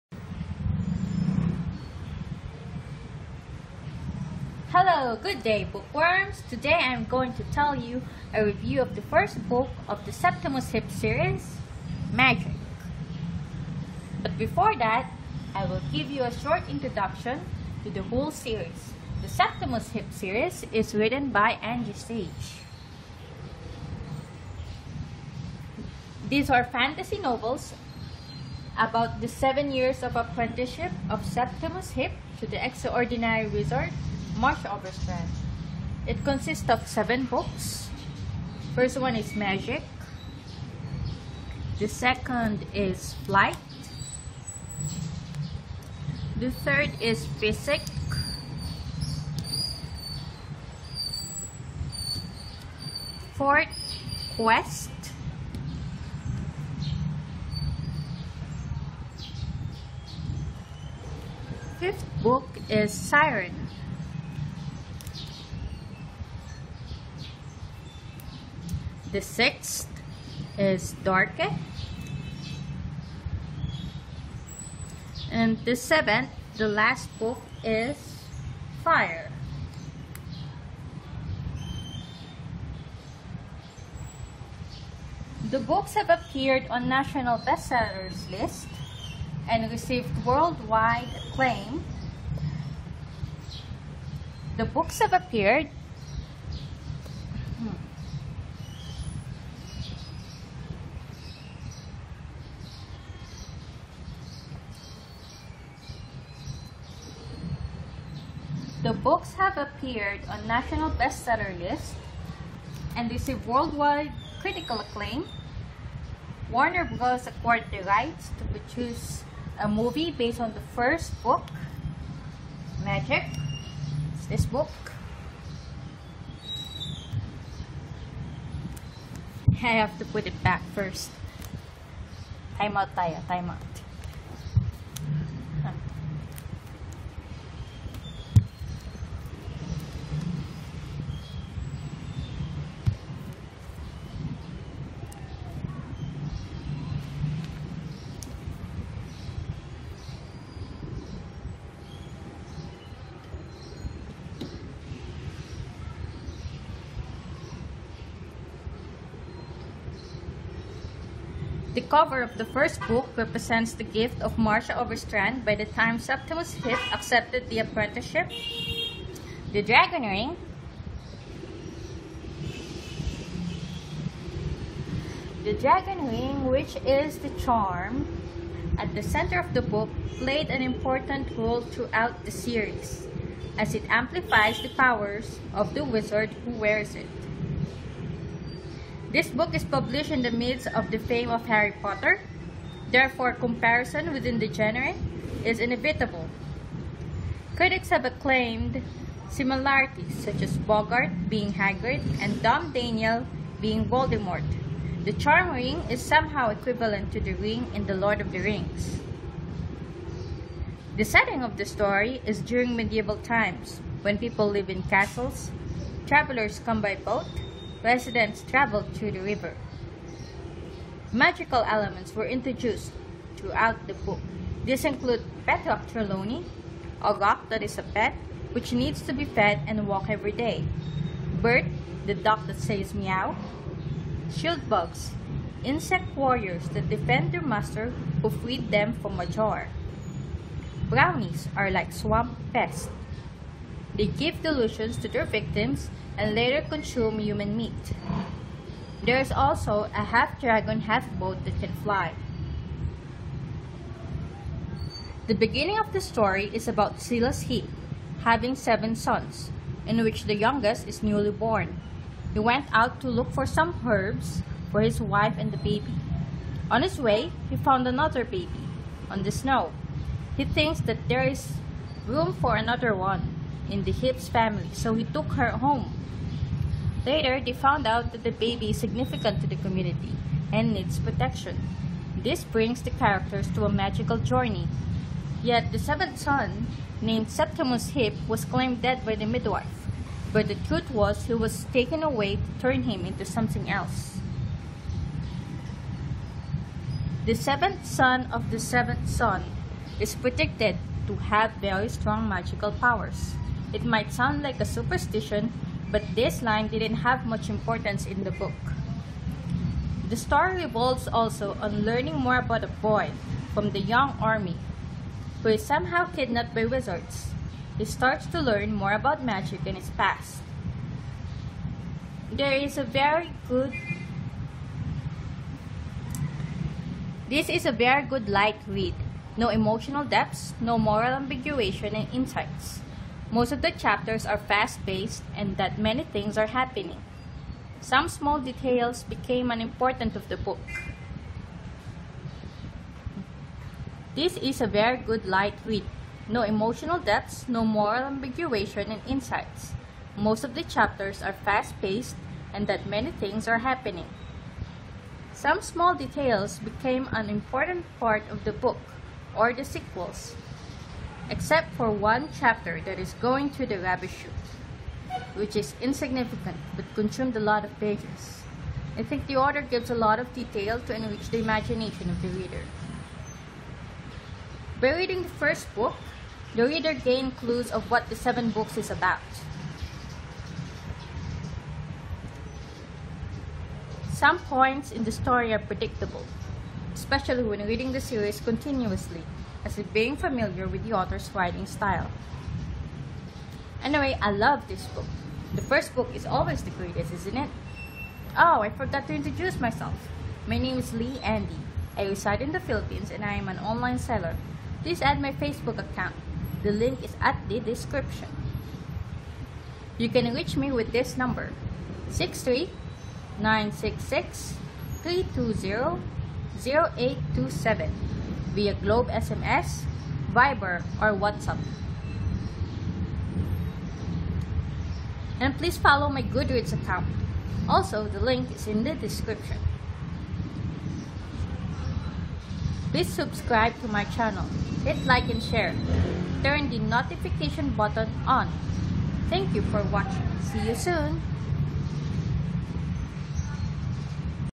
A Review of Magyk Septimus Heap Series Book 1
You can Order the Septimus Heap Series here in Amazon

As a whole set

Hello, good day bookworms! Today I am going to tell you my review of the first book of the Septimus Heap Series, Magyk.

But before that, I will give you a short introduction to the whole series. The Septimus Heap series is written by Angie Sage. These are fantasy novels about the seven years Apprenticeship of Septimus Heap to the Extra-Ordinary Wizard, Marcia Overstrand. It consists of seven books namely; Magyk, Flyte, Physik, Queste, Syren, Darke, and the last, Fyre.

The books have appeared on national bestsellers lists and received worldwide critical acclaim. Warner Bros acquired the rights to produce a movie based on the first book. MAGYK.

The cover of the first book represents the gift of Marcia Overstrand by the time Septimus Heap accepted the apprenticeship. The Dragon Ring which is a charm at the center of the book played an important role throughout the series, as it amplifies the powers of the wizard who wears it.

This book is published in the midst of the fame of Harry Potter, therefore, comparison within the same genre is inevitable. Critics have claimed similarities such as Boggart being Hagrid and DomDaniel being Voldemort. The charm ring is somehow equivalent to the ring in Lord of the Rings.

The setting of the story is during medieval times when people live in castles. Travelers come by boat, residents travel through the river.

Magical elements were introduced throughout the book. These include Petrock Trelawney, a rock that is a pet, which needs to be fed and walk every day. Bert, the duck that says Meow. Shield bugs, insect warriors that defend their master who freed them from a jar. Brownies are like swamp pests, they give delusions to their victims and later consume human meat. There is also a half-dragon, half boat that can fly.

The beginning of the story is about Silas Heap, having seven sons, in which the youngest is newly born. He went out to look for some herbs for his wife and the baby. On his way he found another baby on the snow, he thinks that there is room for another one in the Heaps family so he took her home. Later they found out that the baby is significant to the community and needs protection. This brings the characters to a magical journey. Yet the seventh son, named Septimus Heap was claimed dead by the midwife. But the truth was he was taken away to turn him into something else.

The seventh son of the seventh son is predicted to have very strong magical powers. It might sound like a superstition but this line didn't have much importance in the book.

The story revolves also on learning more about a boy from the young army who is somehow kidnapped by wizards. He starts to learn more about magic and his past.

This is a very good light read, no emotional depths, no moral ambiguation, and insights. Most of the chapters are fast-paced and that many things are happening. Some small details become an important part of the book or the sequels. Except for one chapter that is going through the Rubbish chute, which is insignificant but consumed a lot of pages. I think the author gives a lot of detail to enrich the imagination of the reader.

 By reading the first book, the reader gain clues of what the seven books are about. Some points in the story are predictable. Especially when reading the series continuously, as if being familiar with the authors writing style.

Anyway. I love this book!. The first book is always the greatest, isn't it?

Ohh. I forgot to introduce myself. My name is Li Andy, I reside in the Philippines and I am an online seller. Please add my Facebook account.

And Please follow my Goodreads account.